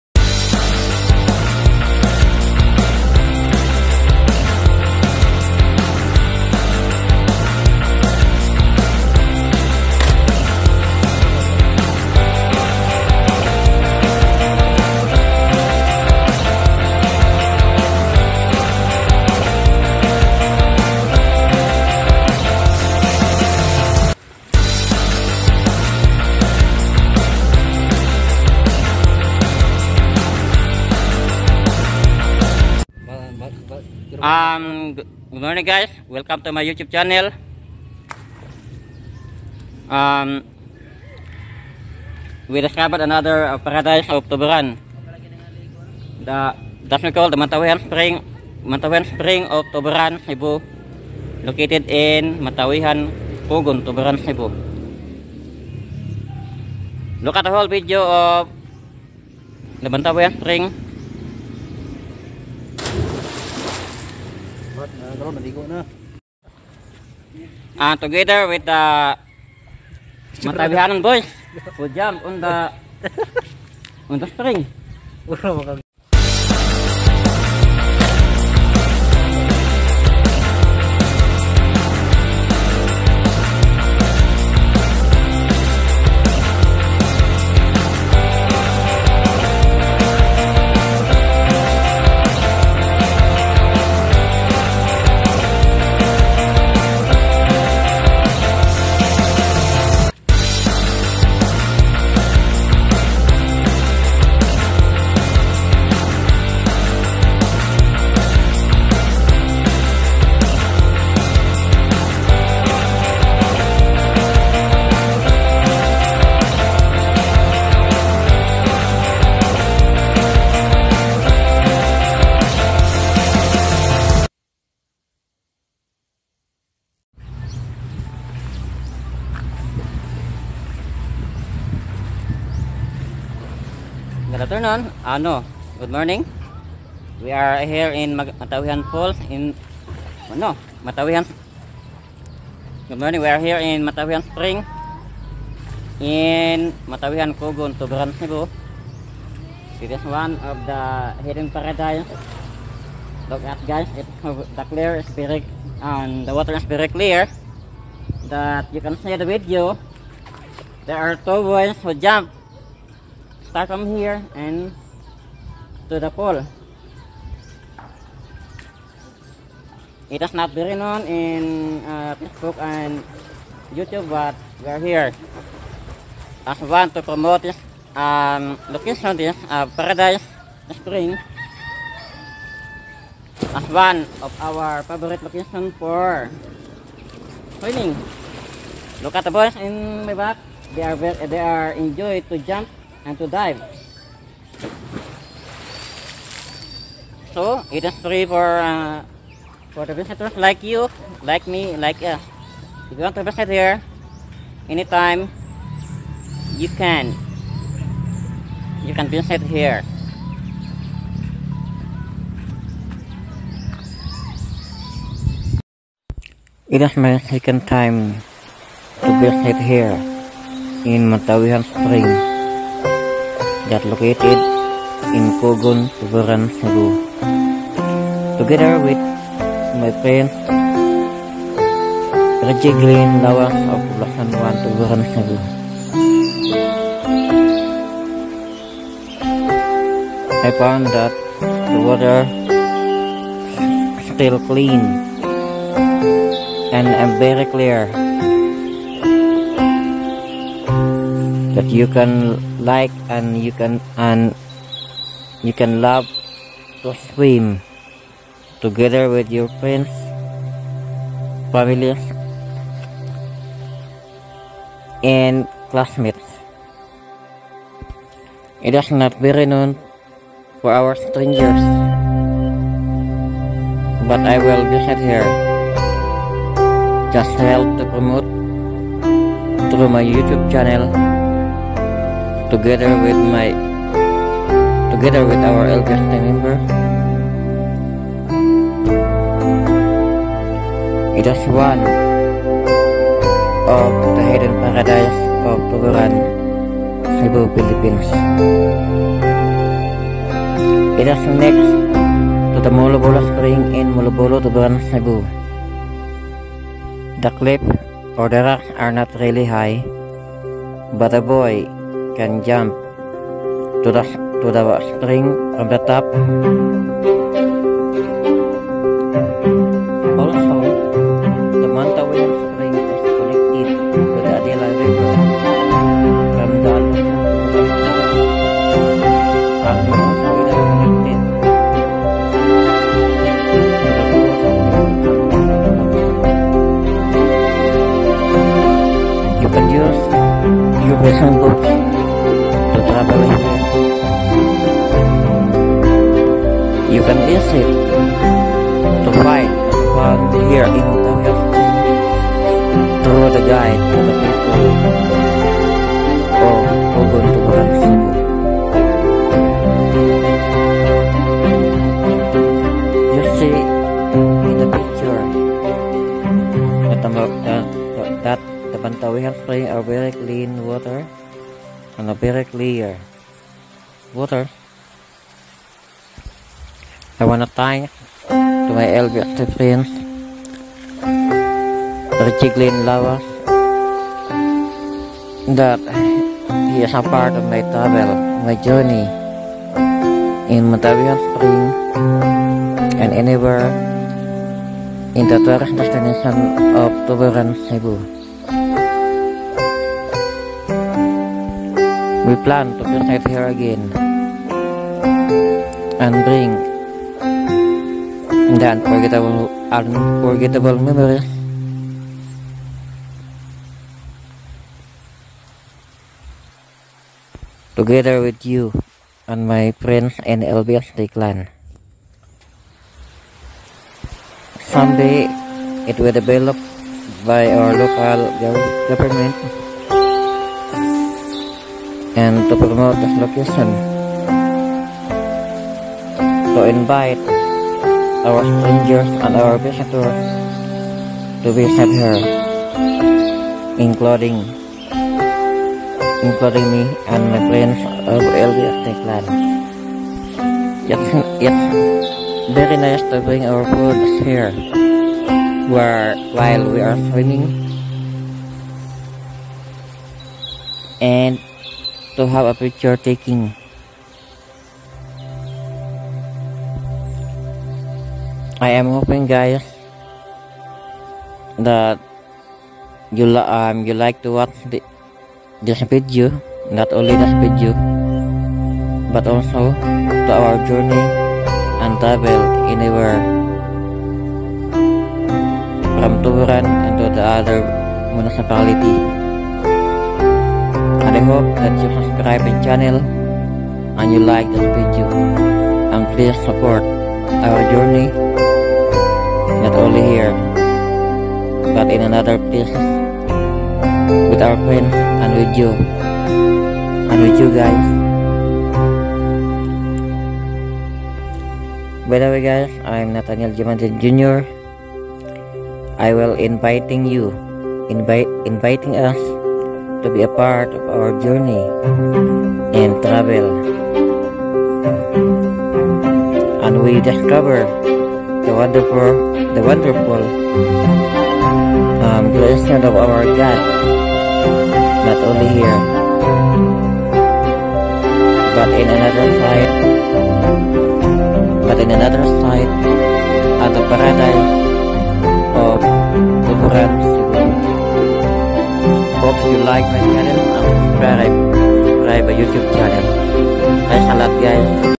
Mantauijan(Mantawehan) Spring, Cogon, Tuburan, Cebu, Philipines Journey With LBHT CLAN.
The other beautiful hot spring that located in Mantawehan Cogon Tuburan Cebu near to the public road.
it has a clear and clean water that match for your dive experienced.